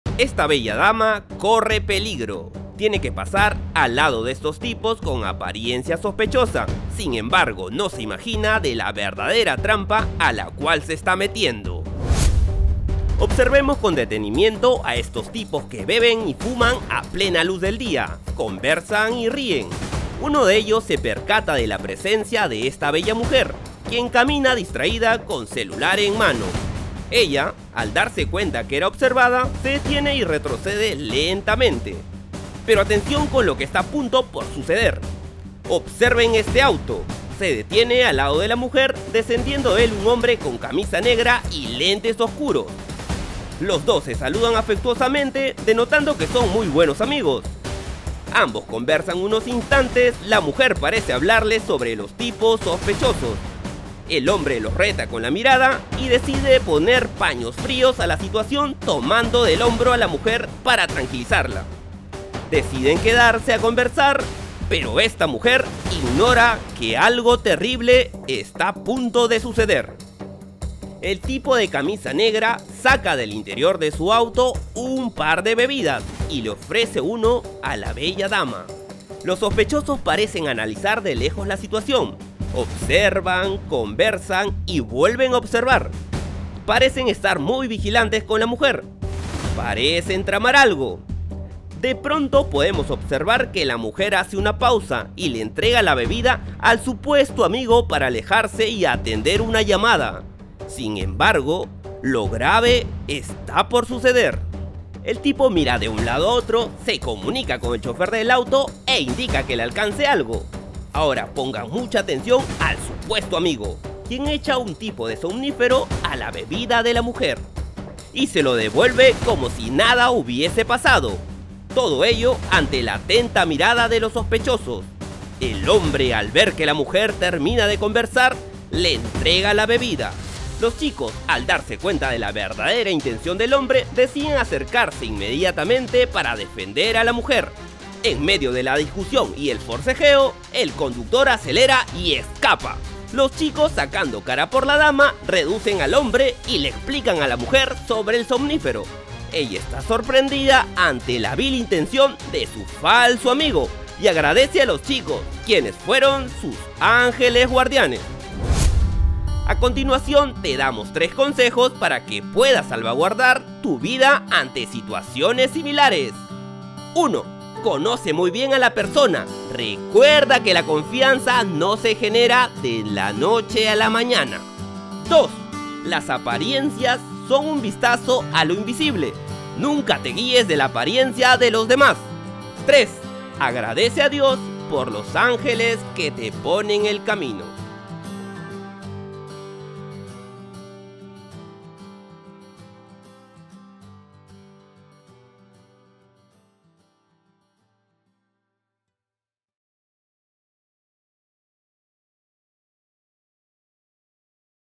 Jamás Juzgues a las personas por su apariencia | La vida te da sorpresas | Agradecer a la Vida
La vida a veces nos toma de sorpresa y para ello debemos de estar atentos a veces parece la persona no es la que parece ,debemos respetar su vestimenta y no juzgar por simples apariencias